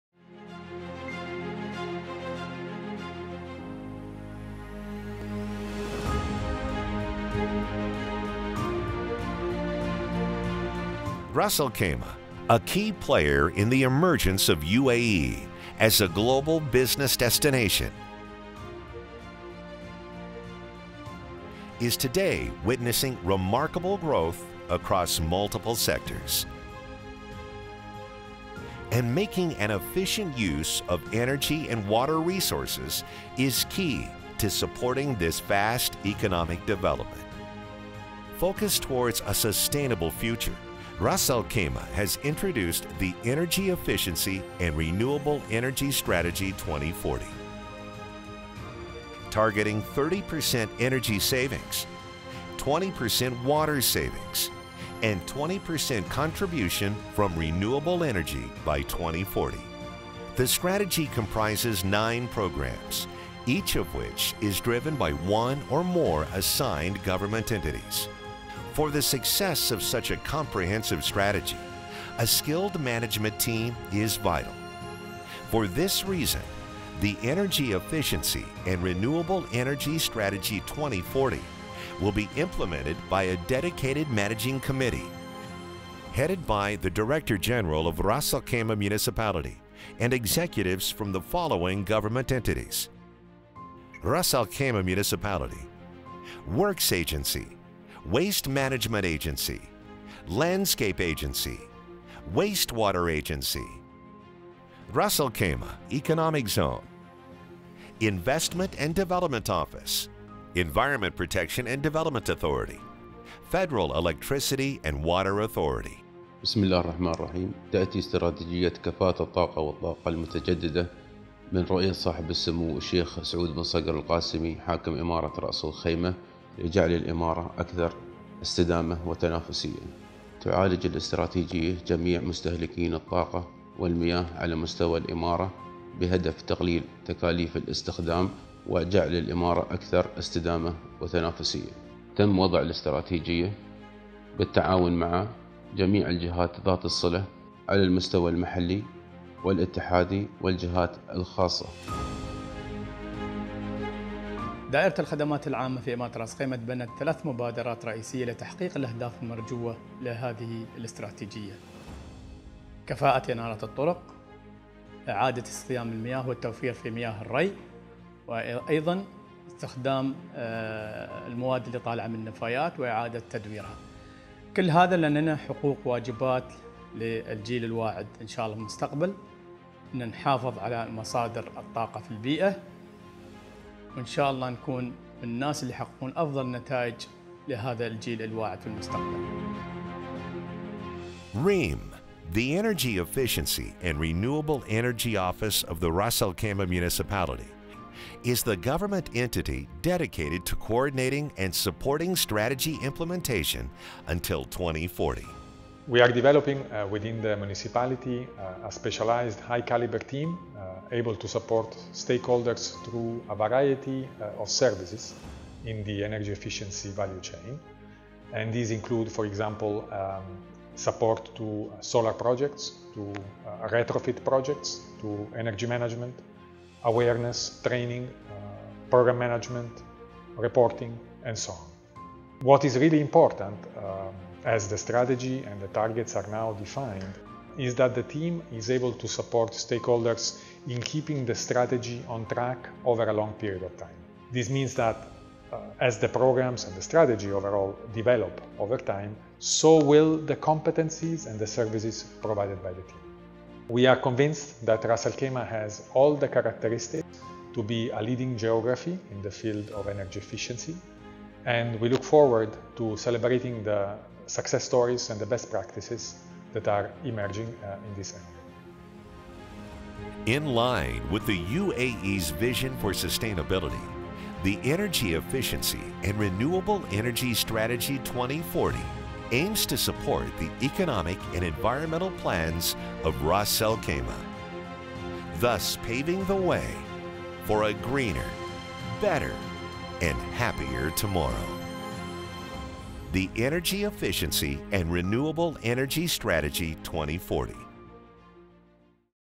Reem (Energy Efficiency & Renewables Administration) ريم ، إدارة كفاءة الطاقة و الطاقة المتجددة
أطلقت دائرة بلدية رأس الخيمة مؤخراً برنامج بارجيل، شروط المباني الخضراء في إمارة رأس الخيمة و الذي يضع الحد الأدنى من المعايير لجميع المباني الجديدة في رأس الخيمة. و من المتوقع أن توفر المباني المطابقة لبرنامج بارجيل ما يقارب 30٪ من قيمة الطاقة والمياه مقارنةً مع المباني الاعتيادية في الإمارة. تم اعتماد برنامج بارجيل من قبل المجلس التنفيذي في الإمارة، و من ثم تم إطلاق المرحلة التطوعية للبرنامج في 29 يناير 2019. إن دائرة بلدية رأس الخيمة تُشجع أصحاب المباني والمطورين العقاريين على تطبيق برنامج بارجيل على مشاريعهم الجديدة من خلال توفير الحوافز للمتبنين الأوائل خلال المرحلة التطوعية على شكل خصومات في رسوم رخص البناء النهائية.

Barjeel, the Green Building Regulations of Ras Al Khaimah was recently launched by Ras al Khaimah Municipality. The regulations set minimum sustainability standards for all new buildings in Ras Al Khaimah. Buildings in line with Barjeel are expected to consume 30% less energy and water with minimal impact on construction costs.
Barjeel was approved by the Executive Council of Ras Al Khaimah and a one-year voluntary phase started on January 29, 2019. RAK Municipality encourages building owners & developers to apply Barjeel to new projects and provides incentives to early adopters during the voluntary phase in the form of discounted final building permit fees.